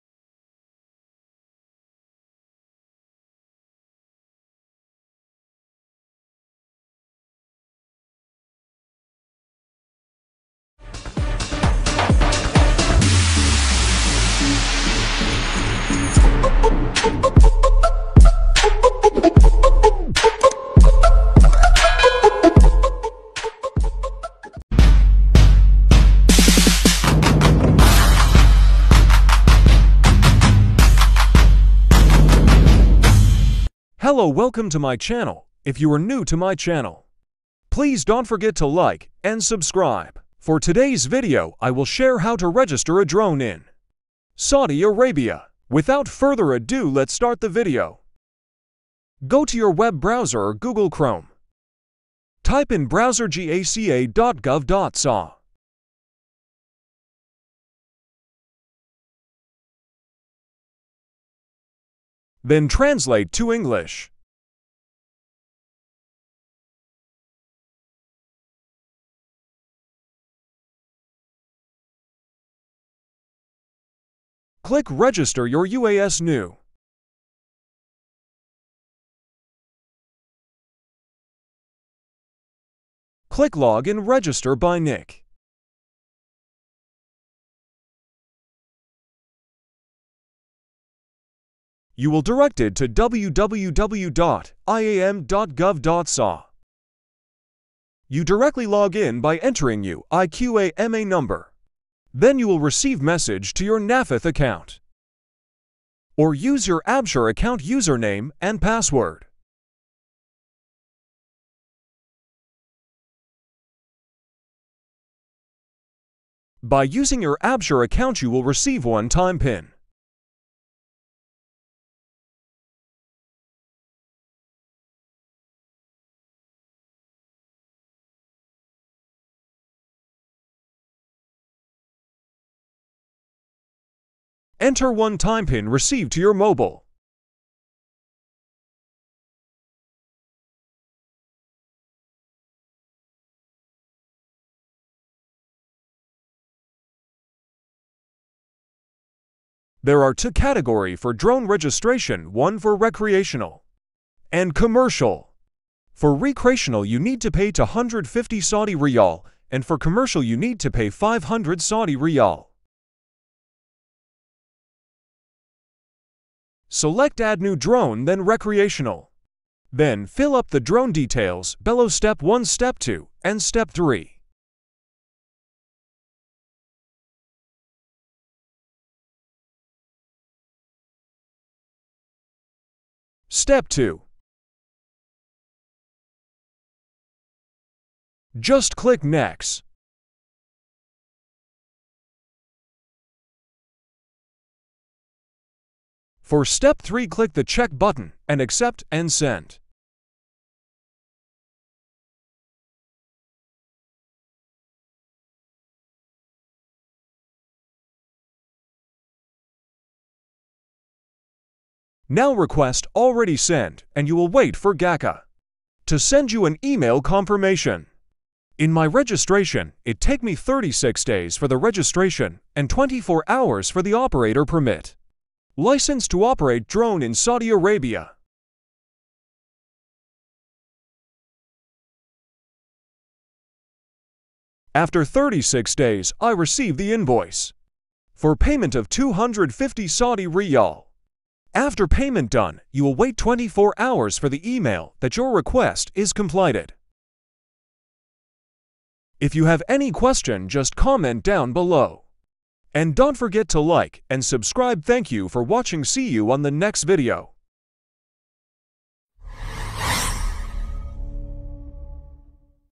HOW TO REGISTER A DRONE IN SAUDI ARABIA | PAANO MAGREGISTER NG DRONE SA SAUDI ARABIA |WildredTV
GACA Drone permits December 2023
gaca.gov.sa

How to get a Drone License in Saudi Arabia.
Drone Registration process.

1.Open browser gaca.gov.sa
 A. Use absher account for login and new         registration GACA UAS login account.

2.Register a new drone.
A.Recreational
B.Commercial

3.Fill up the form (Drone details.)
A. After registration succesfully sent.
B.Wait email confirmation of GACA for invoice payment registration.It takes me 36 days to receive the invoice.
C.Recreational - 250SR.
D. Commercial - 500SR.
E.Payment process is thru SADAD.

4.After payment of registration.Wait for 24hrs for the confirmation.

5 Print out registration certificate.

Drone License Process.
1.After registration approve.
2.Apply Operator permit click new license.
3.Fill up the form.
4.After succesfully sent.
5.Wait for 24hrs for the permit.

Done.